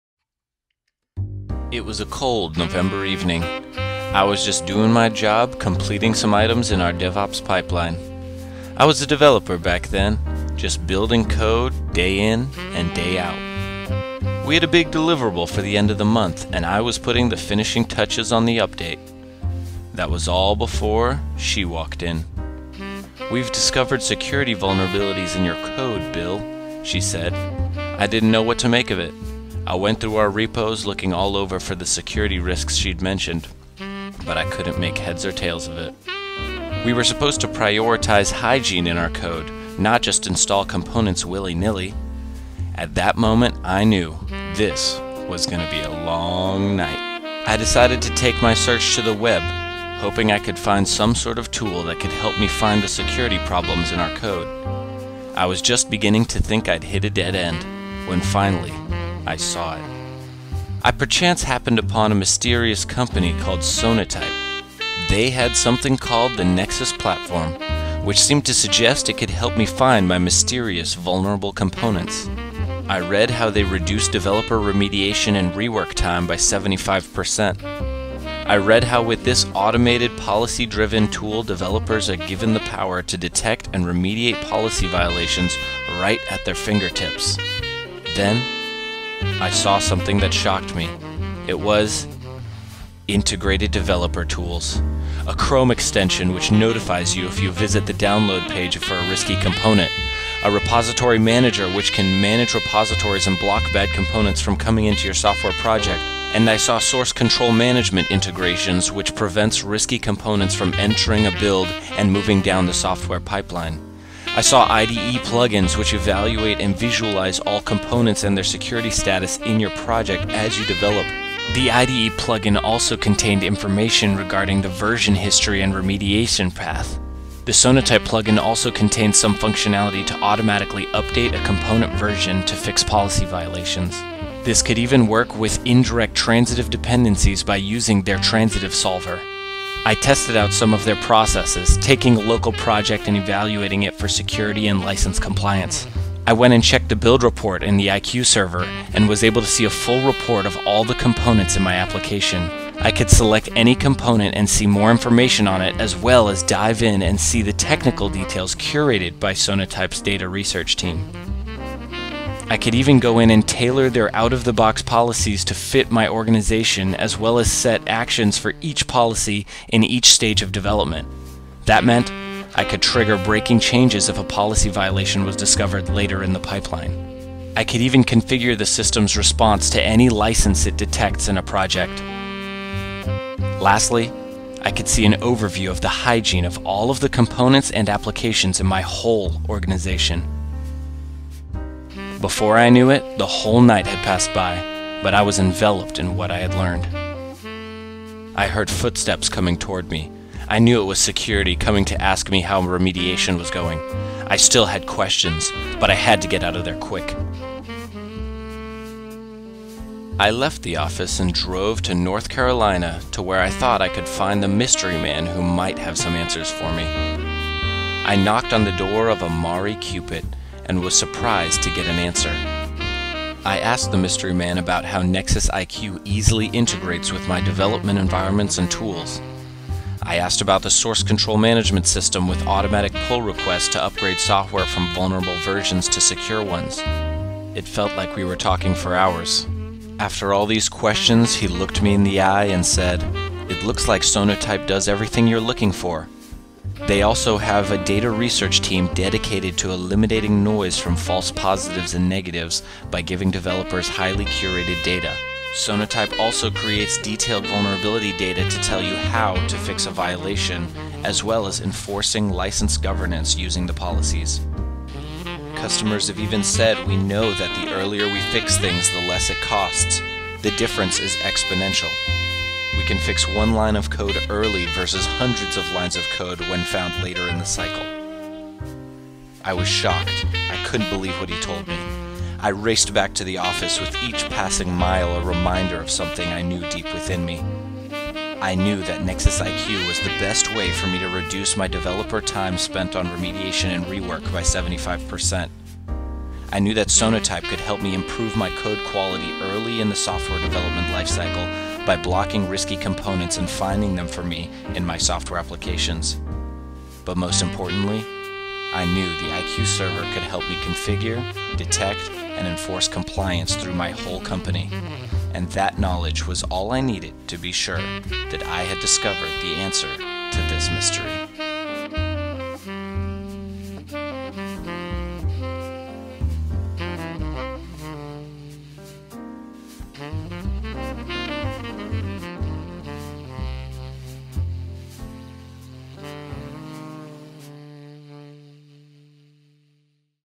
Mystery Solved: Sonatype Makes Developers' Lives Easier
Just as Bill was hitting his stride, completing items in his orgs DevOps' pipeline ahead of a big deliverable at the end of the month - he was stalled by security with a list of open source vulnerabilities in his code. Looking high and low in his repos, Bill still couldn’t find the mysterious vulnerabilities security told him about. It was then, he knew, he needed a better way.

He quickly realized the Sonatype Nexus platform solved all of his problems with integrated developer tools - a way to find all of the issues and vulnerabilities within open source code, before security even got a chance to check. Watch Bill’s story and see if Sonatype can make your life easier too.